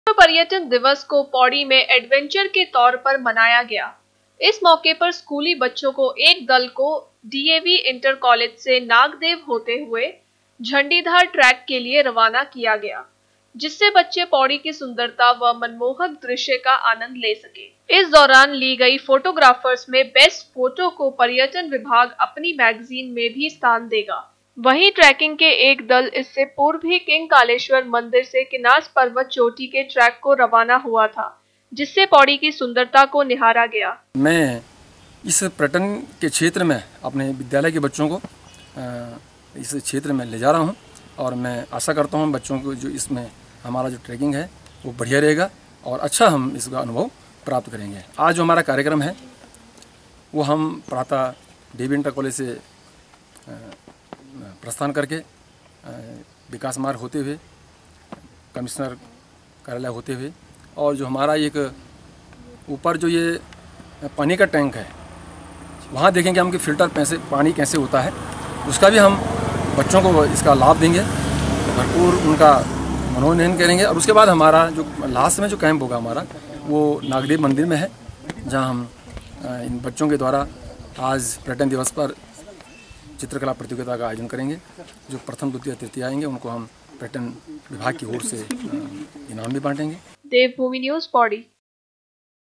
विश्व पर्यटन दिवस को पौड़ी में मनाया गया एडवैंचर के तौर पर
विश्व पर्यटन दिवस को पौड़ी में एडवैंचर के तौर पर मनाया गया इस मौके पर स्कूली बच्चों को एक दल को डीएवी इंटर कालेज से नागदेव होते हुए झंडीधार ट्रैक के लिये रवाना किया गया